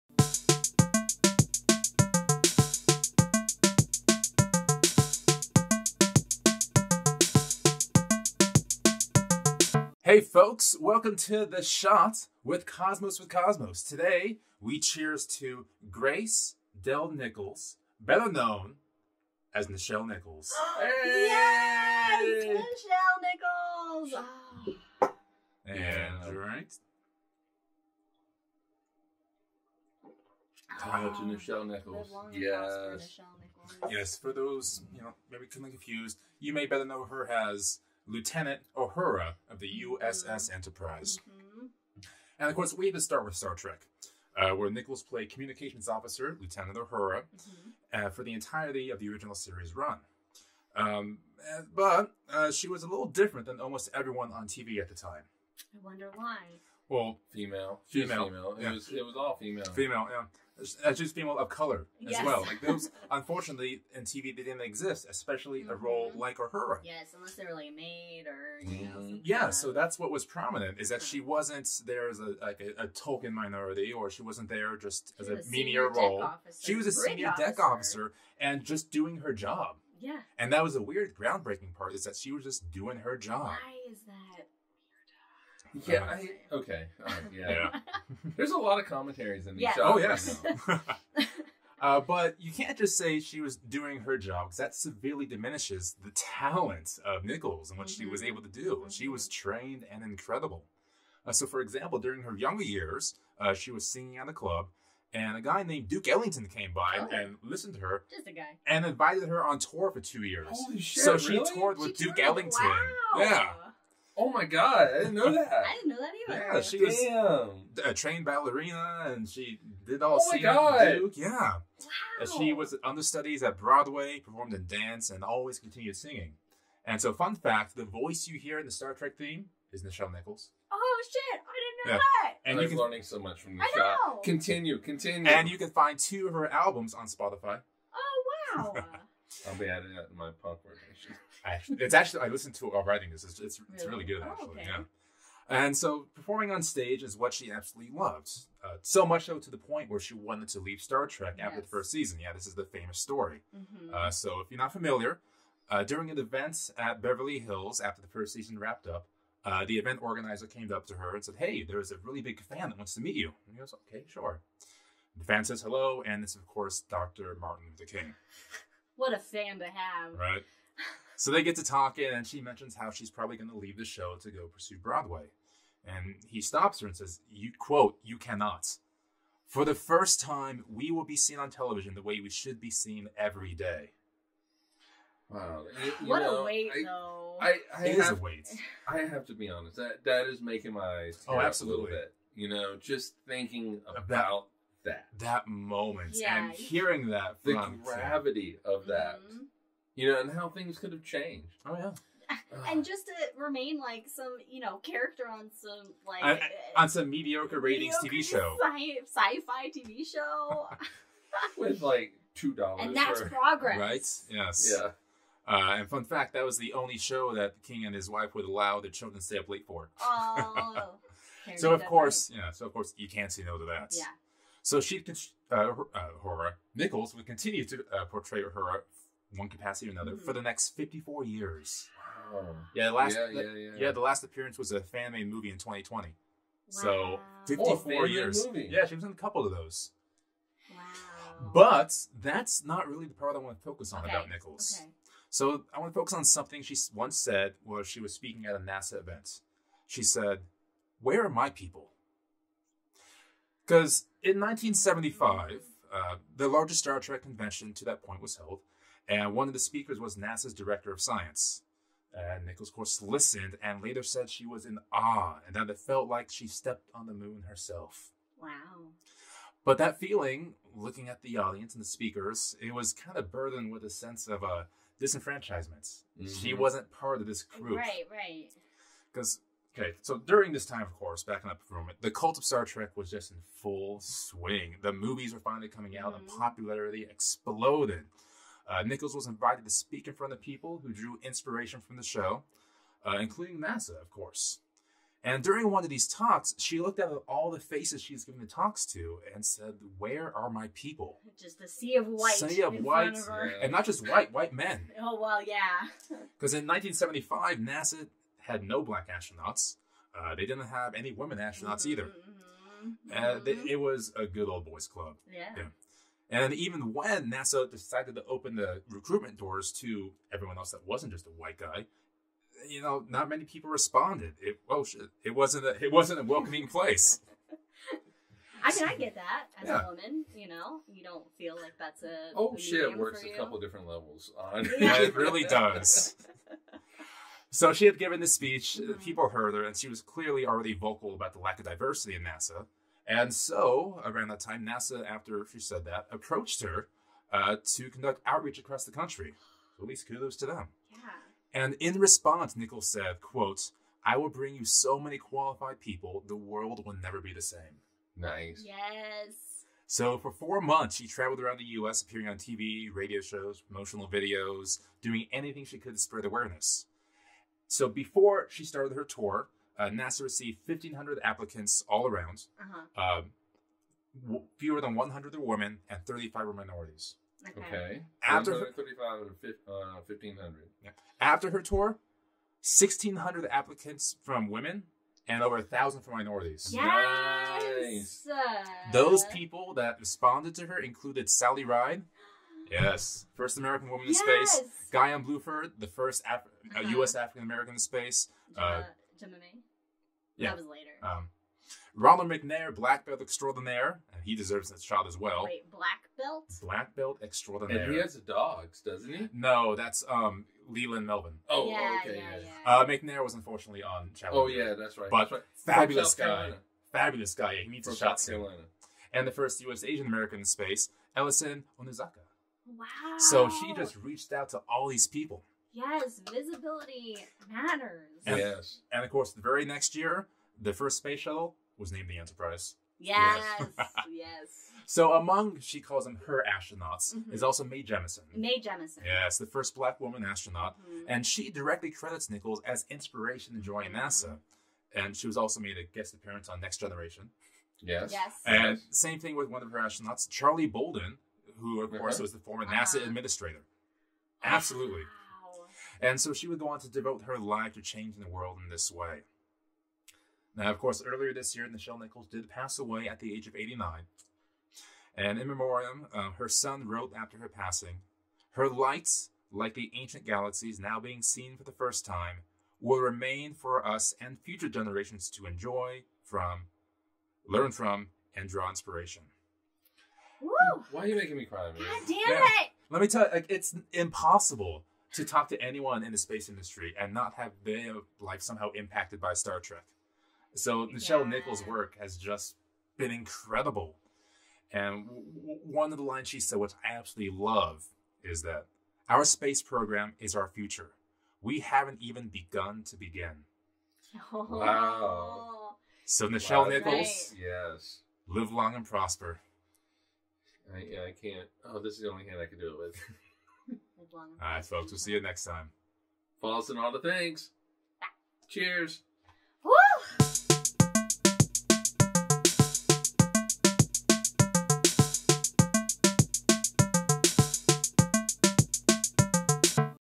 The SHOT with CwC: Nichelle Nichols - Live Long and Prosper!
Welcome to the SHOT with CwC where we all take a shot and tell a brief story about one of the universe's many topics!

In this episode, Brandon talks honors Nichelle Nichols and her constructions to diversity in space!

*Always Drink Responsibly*